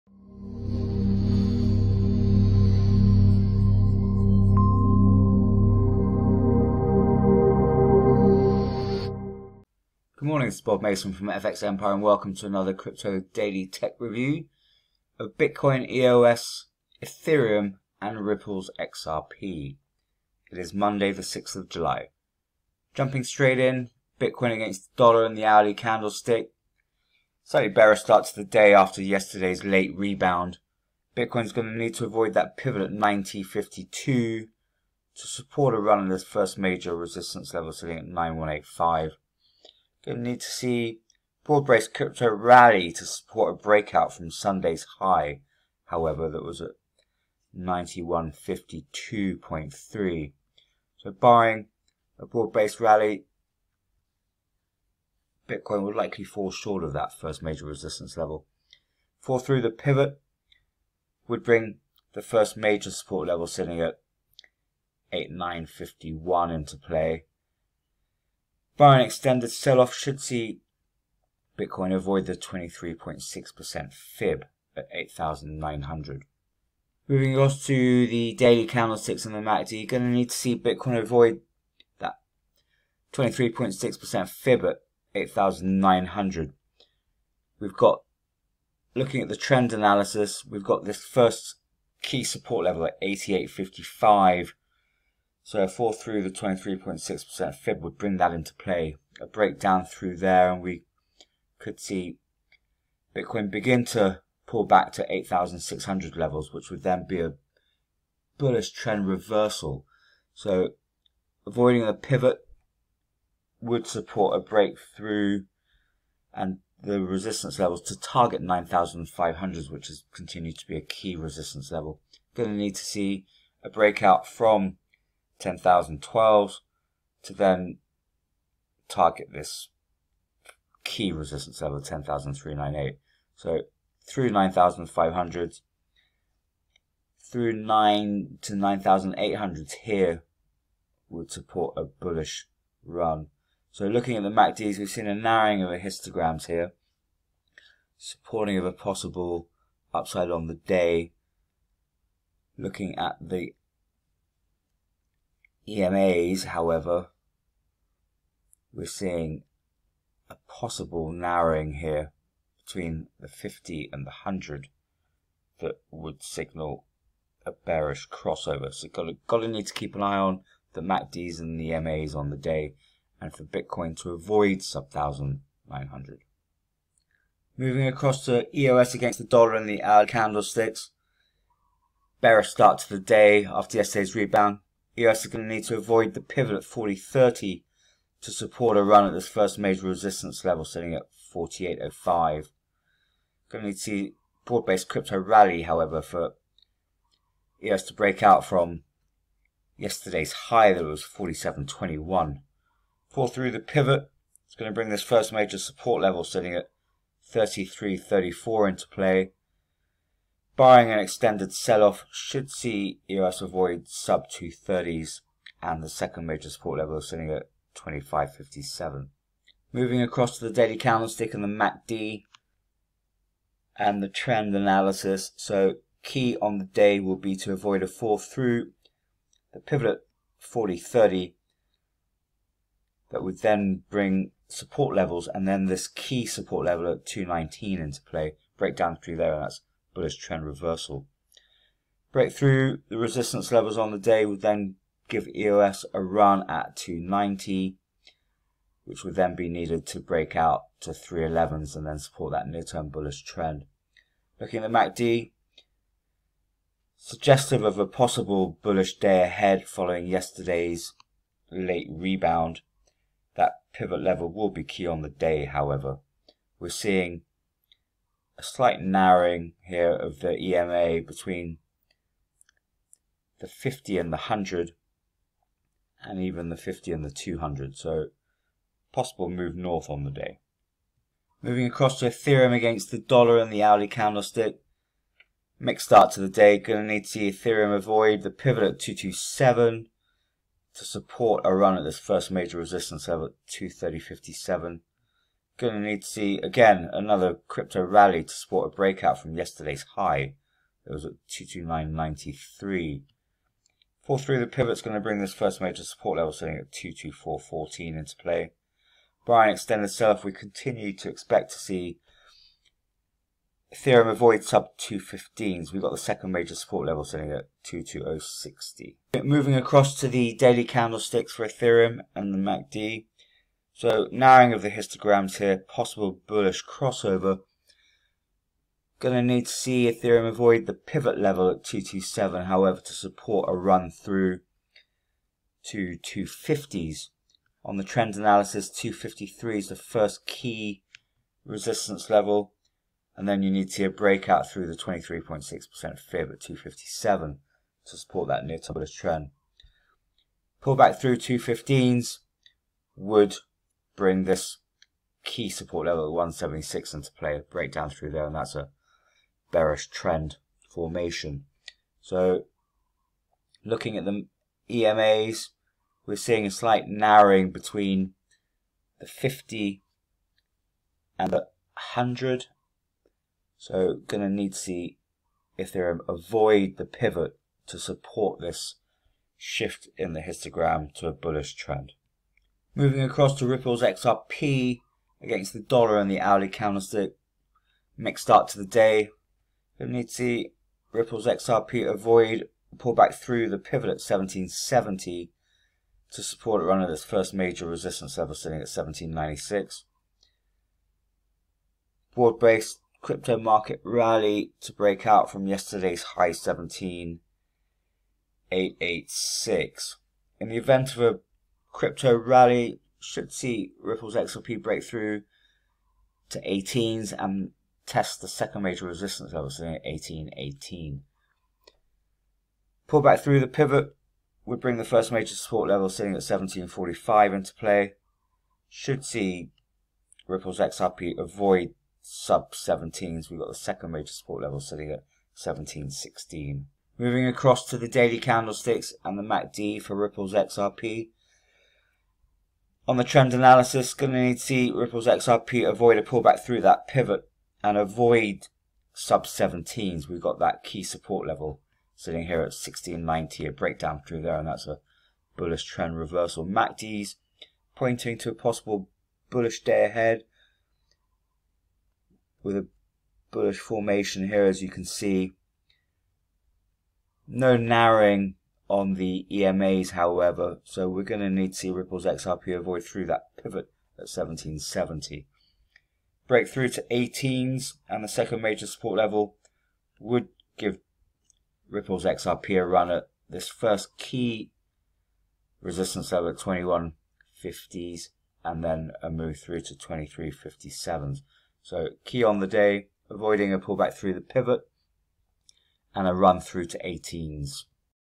Bitcoin, EOS, Ethereum, and Ripple's XRP Daily Tech Analysis July 6th, 2020
It's a mixed start to the day for the majors. A fall through the day's pivot levels will bring support levels back into play.